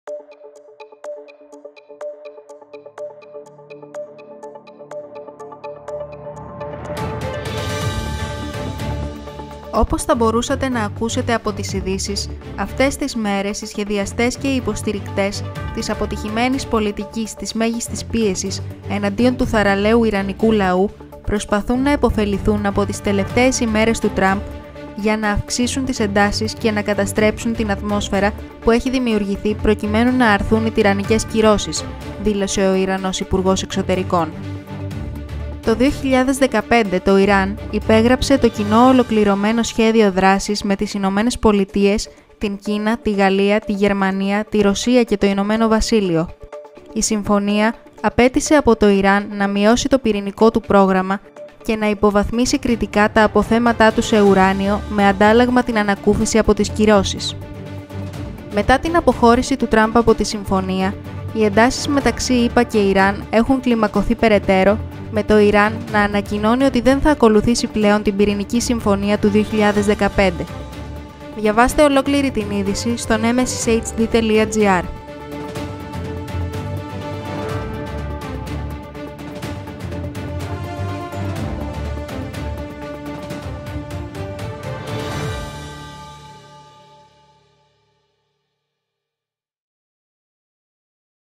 ΑΠΑΝΤΗΣΗ ΙΡΑΝ ΣΤΟΥΣ ΕΧΘΡΟΥΣ ΤΟΥ - "Προσπαθούν να διατηρήσουν τις κυρώσεις εναντίον μας" - ΕΙΔΗΣΕΙΣ
Ευχαριστώ για την στήριξή σας, σας περιμένω στο επόμενο βίντεο!